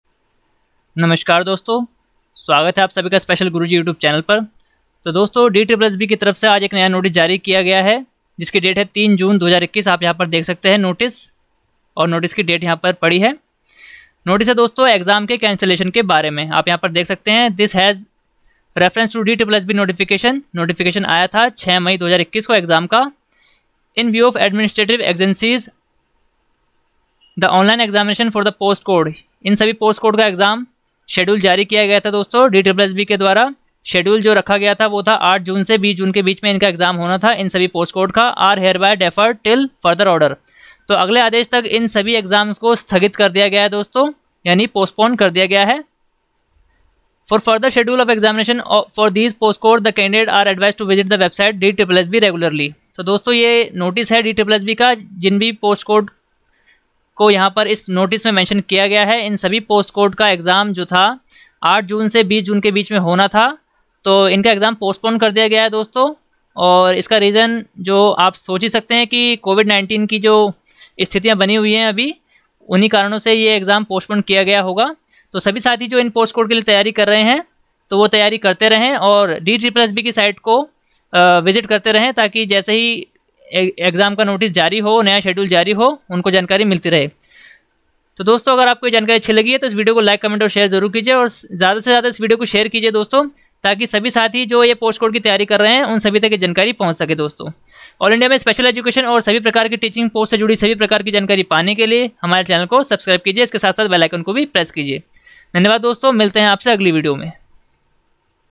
DSSSB PGT EXAM POSTPONED|JUNE 2021 EXAM CANCEL BY DSSSB|DSSSB NEW NOTICE|ALL POST CODE EXAM Deferred
DSSSB Online examinations scheduled w.e.f. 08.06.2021 to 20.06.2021 are hereby deferred till further order DOWNLOAD NOTICE